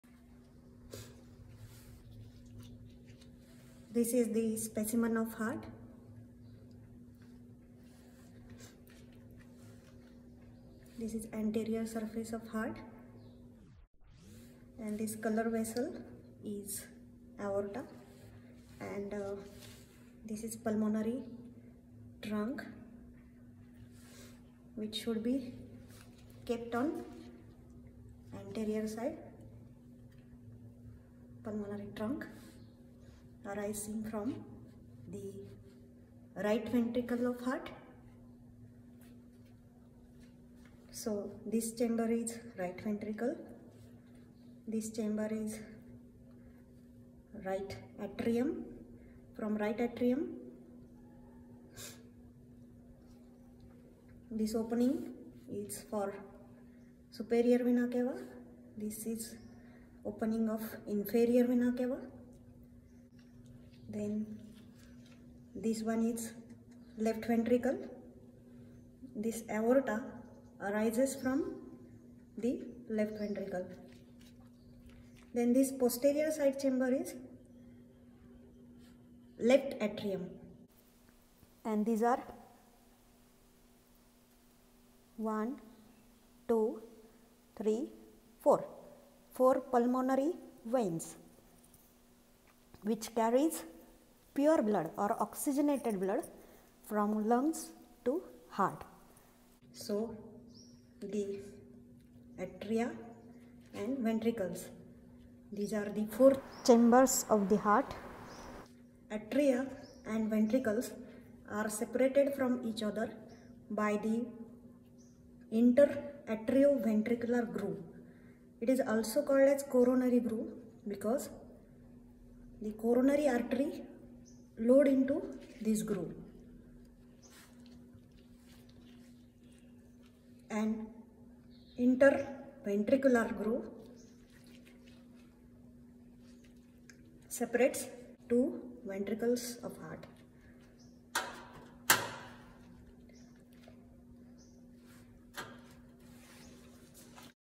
Heart external features - part 1
Heart By Dr Karle Geetanjali, Professor, PMT's  college, Shevgaon.

This video shows external features of the heart. It shows great vessels attached to the various chambers of the heart.
Heart is made up of 4 chambers- 2  Atria and 2 ventricles.
1) The right Atrium is receiving chamber of deoxygenated blood from superior and inferior vena cava.
2) Right ventricle is the chamber located at the right inferior side of the heart. Pulmonary trunk arises from right ventricle.
3) Left ventricle forms apex of the heart. It is left inferior chamber of the heart. Ascending aorta starts from left ventricle of the heart.
4) Left atrium forms major portion of the base of the heart and it is left upper sided chamber located at the posterior side. Four pulmonary veins opens into left atrium.
Grooves - Atrioventricular groove separates atria from ventricles it is also called as coronary groove. Because it’s anterior right sided part lodges Right coronary artery.
Interventricular groove separates two ventricles.